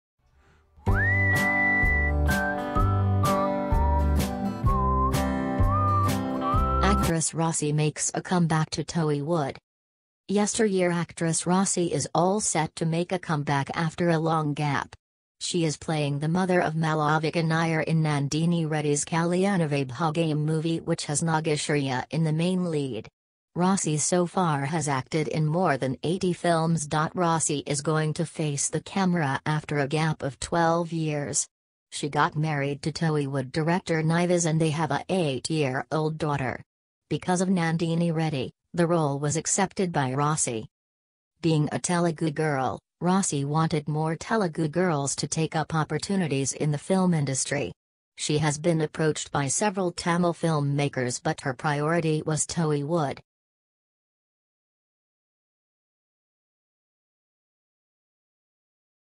Actress Raasi Makes A Come back to Tollywood
Kalyana Vaibhogame is the first film that the actress has accepted after marriage.Yesteryear leading actress Raasi is all set to make a comeback after a long gap. The actress, who has starred in more than 80 films as the female lead, is playing Malavika Nair’s mother in Kalyana Vaibhogame to be directed by Nandini Reddy.“I am doing this film because of Nandini Reddy. She had come to me long ago for this role. I had told her that I was not interested and also, my daughter was just three months old and I had to look after her.